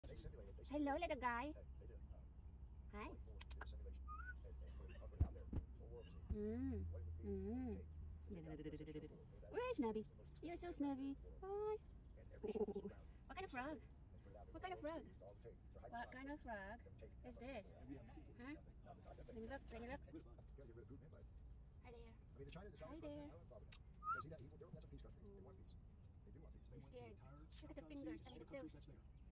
in my camp chair
this little creature sit right close. to me having convo. shortvideo wild frog in my camp chair